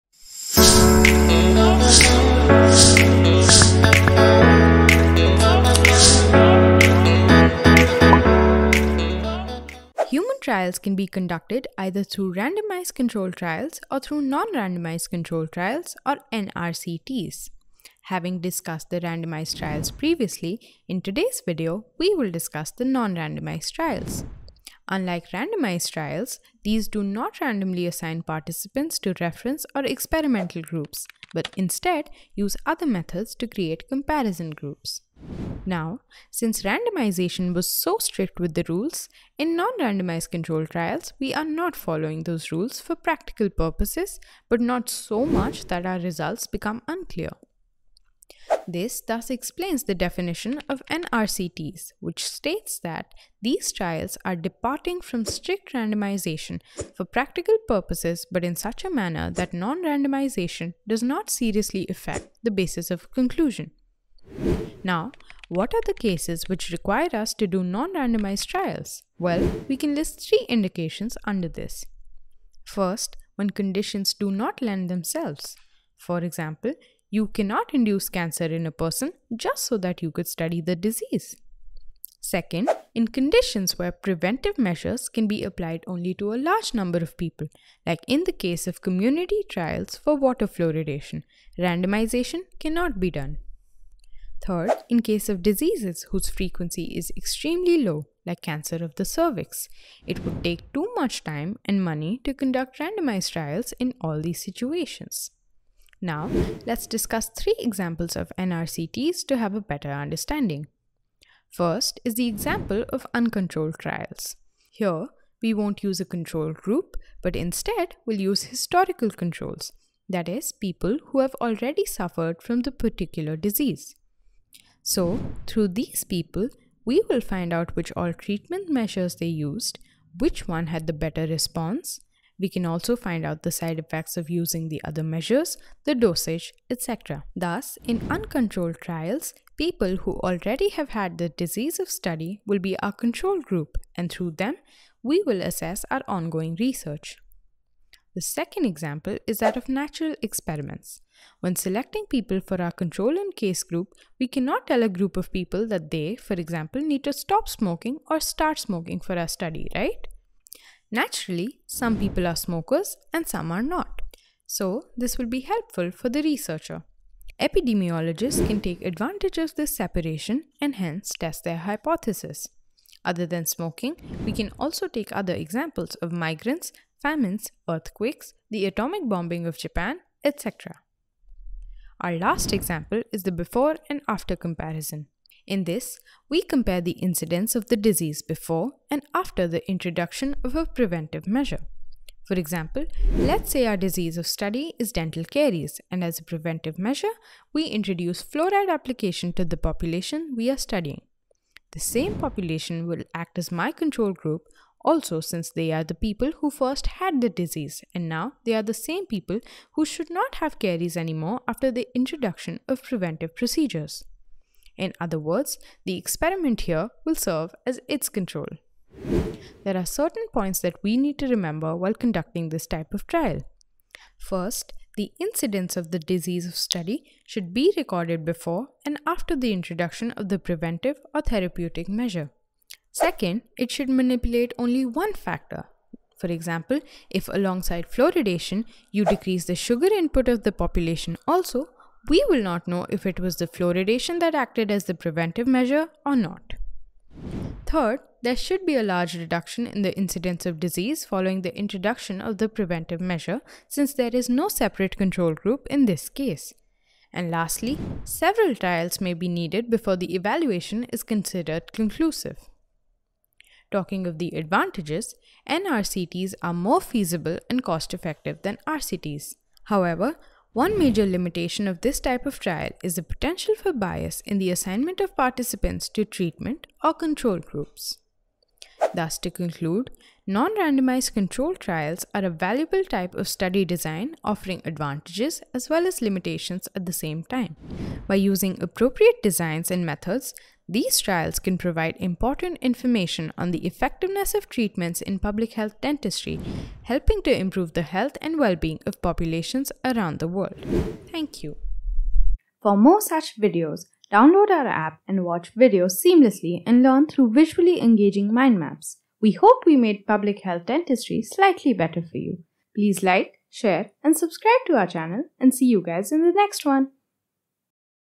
Non-Randomized Trials | Indications, Examples, Advantages & Limitations |Public Health Dentistry
Welcome to our gripping dive into the world of Non-Randomized Clinical Trials (NRCTs)! In this video, we'll unpack the "Introduction to non-randomized trials", shedding light on their pivotal role in medical research.Ever wondered about the "Indications for non-randomized trials"? We’ll explore key scenarios where these trials shine.  Plus, get ready for real-world insights as we share "Examples of non-randomized trials" that have transformed patient care! Lastly, we’ll weigh the "Advantages and limitations of NRCTs", giving you a comprehensive overview of their impact.

Time stamps:
0:11 - Introduction to non-randomized trials
1:07 - Indications for non-randomized trials
1:48 - Examples of non-randomized trials
4:27 - Advantages and limitations of NRCTs